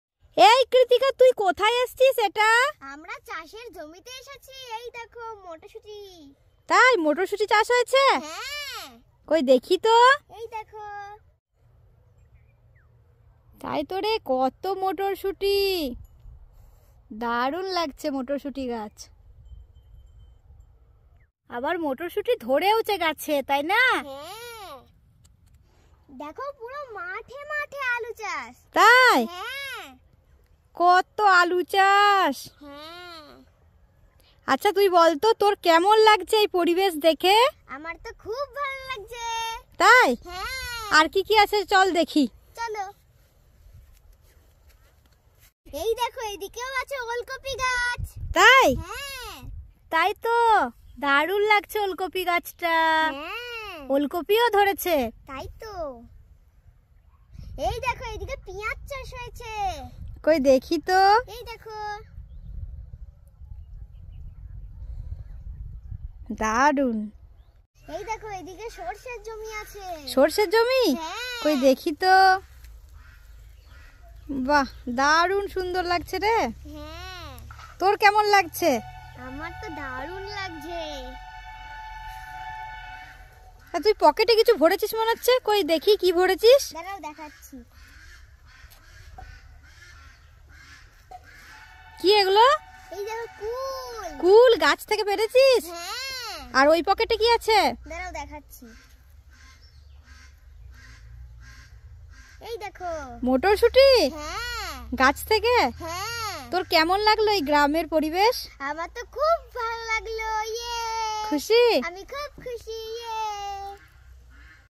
কৃতিকা এটা কোথায় ঘুরতে এসেছে আর এই গ্রামের পরিবেশ কৃতিকার কেমন লাগলো@Kritika Channel
কৃতিকা এটা কোথায় ঘুরতে এসেছে আর এই গ্রামের পরিবেশ কৃতিকার কেমন লাগলো